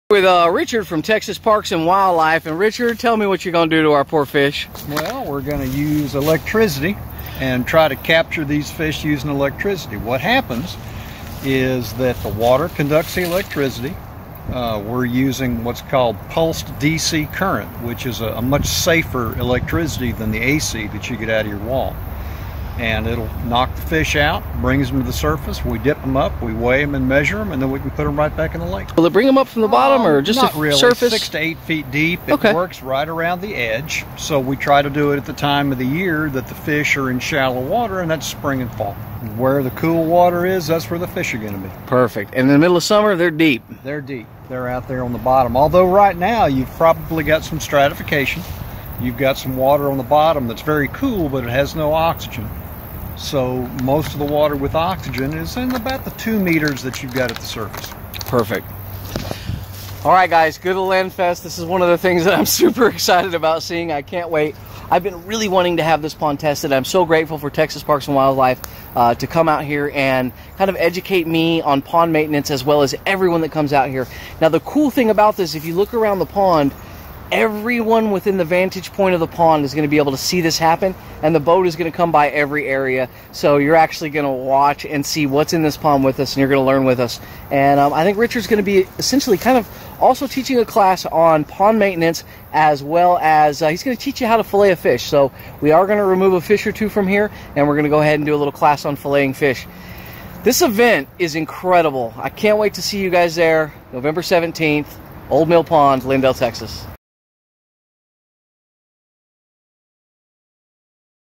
What this GOVERNMENT AGENCY is doing is "SHOCKING"!...literally
The good of the land fest is going to be awesome! Join Texas Parks and Wildlife while they deploy the fish shocking boat and count the fish in our pond.